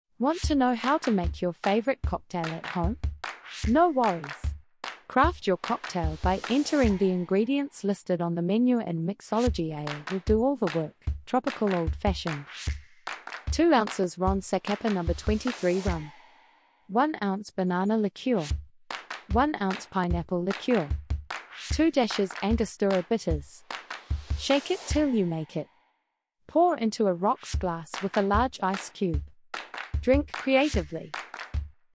Tropical Old Fashioned - Mixology AI
🍹 Recreating my Favorite Restaurant Drink at Home! 🏠✨
When I discovered the Tropical Old Fashioned at my favorite restaurant, I knew I had to bring that vacation vibe to my own space! 🌴🌺 Thanks to the Mixology app, I transformed my kitchen into a tropical paradise and concocted this divine cocktail. 📱🍹
With precise measurements and step-by-step instructions, the Mixology app made it a breeze to recreate the magic. 🍌🍍🥃 Now, I can sip on the tantalizing blend of Ron Zacapa No. 23 Rum, Banana Liqueur, Pineapple Liqueur, and Angostura Bitters whenever I please! 🌈✨
Join me in raising a glass to indulgence and imagination! 🥂🔥 Share your favorite restaurant-inspired creations and tag @MixologyApp for a virtual toast. Cheers to bringing the taste of the extraordinary home! 🍹🏡💫